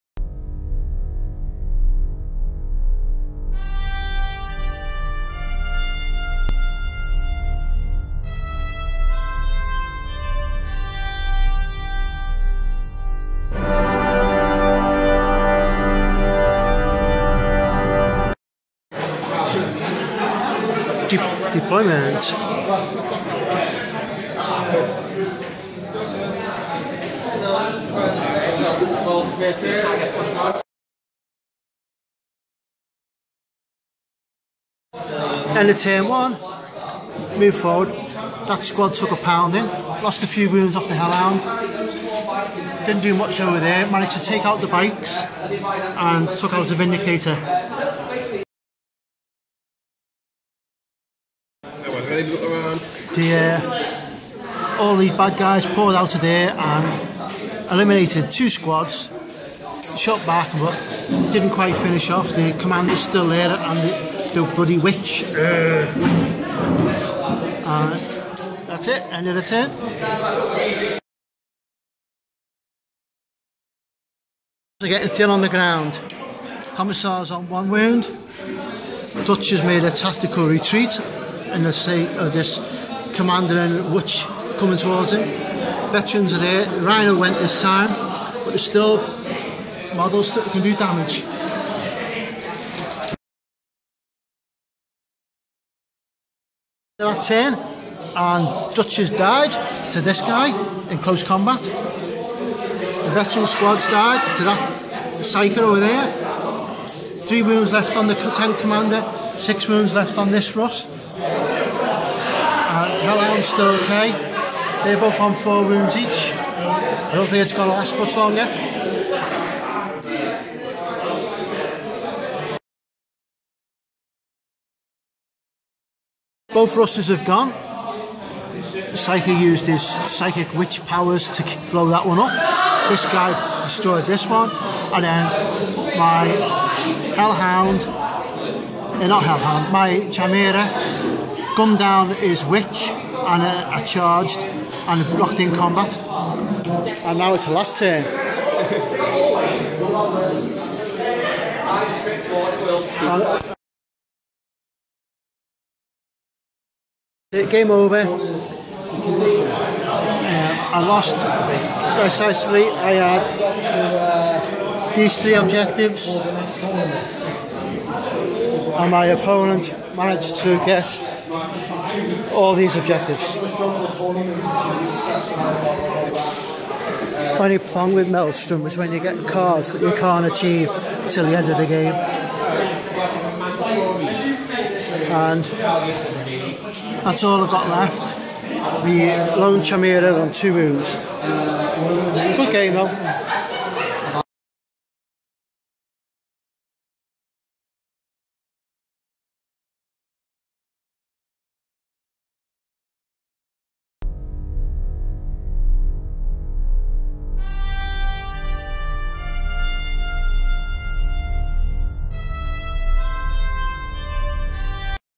Cadians vs Death Guard - Warhammer 40,000 Battle Report 1000pts
Short form Battle report, were my 123rd Cadian face off against the forces of evil in the form of traitor Marines the Death Guard.